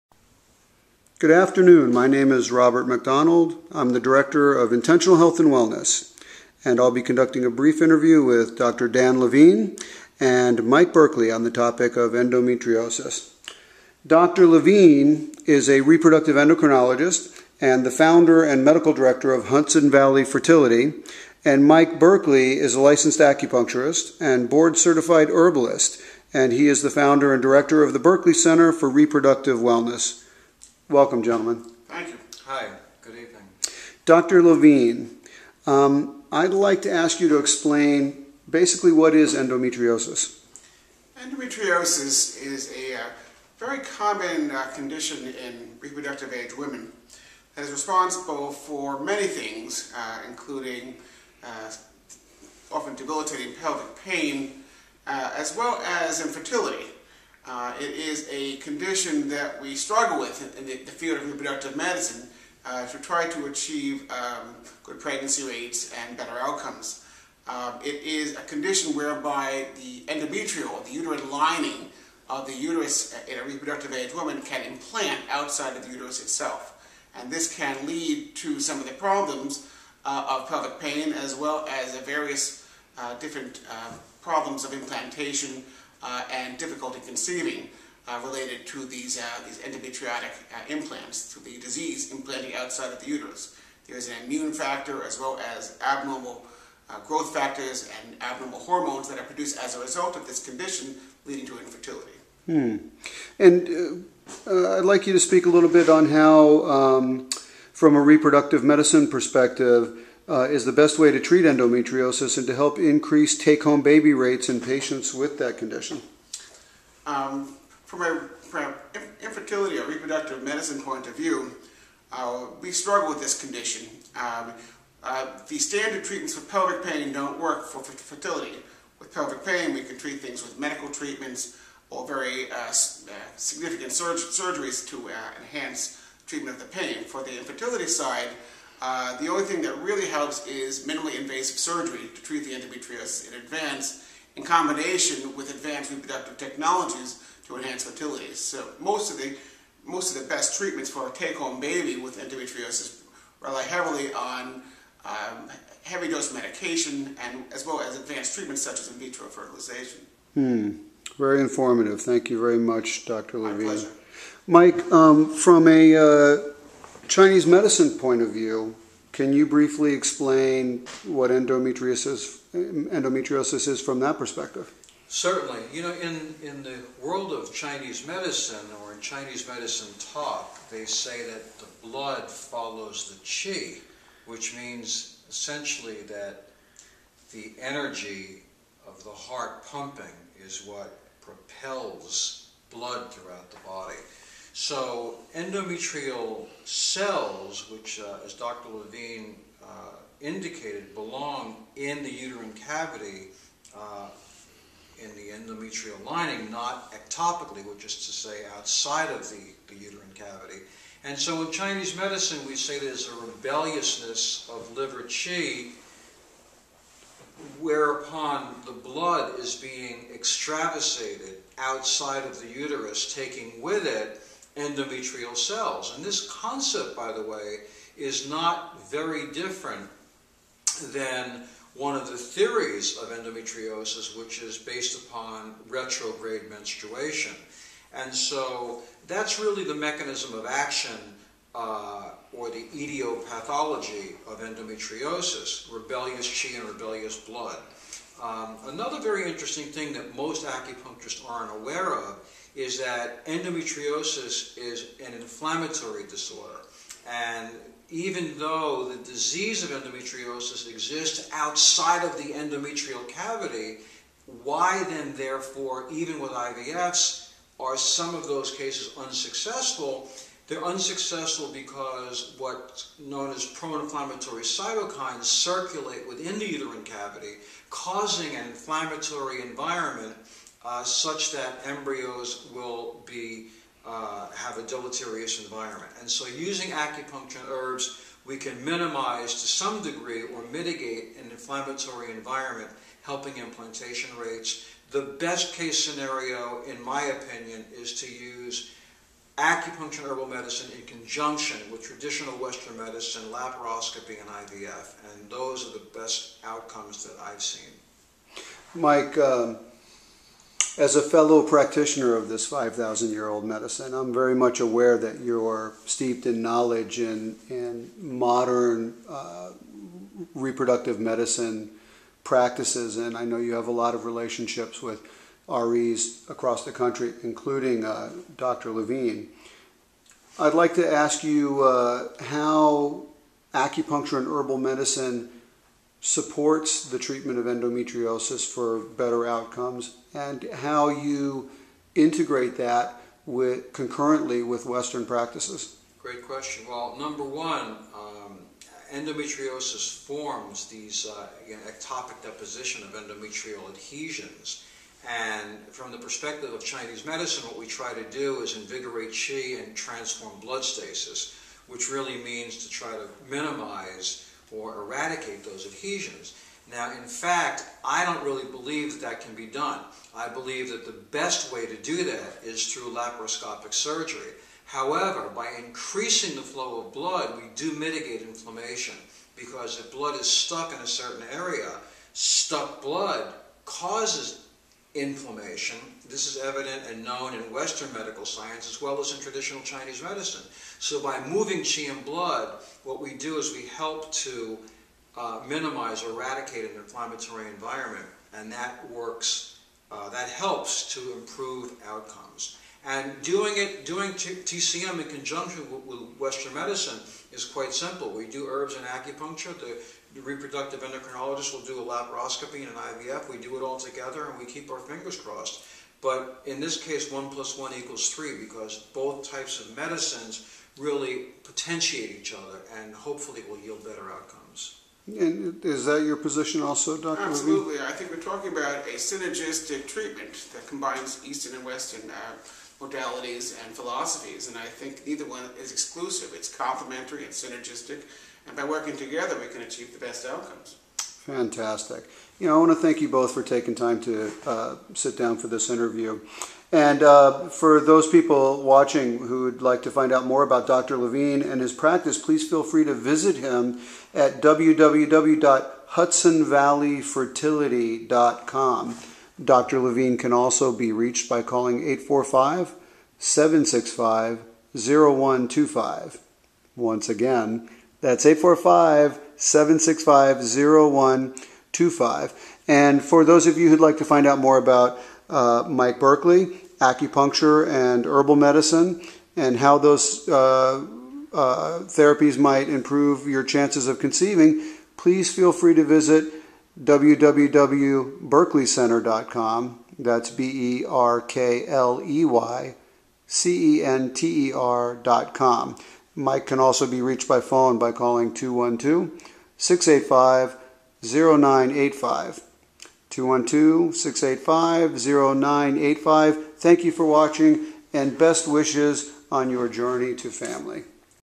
Endometriosis - An Eastern and Western Approach
Dr. Dan Levine, reproductive endocrinologist and Mike Berkley, Licensed Acupuncturist and Herbalist discuss how to diagnose and treat endometriosis with Western medical approaches, acupuncture and herbal medicine.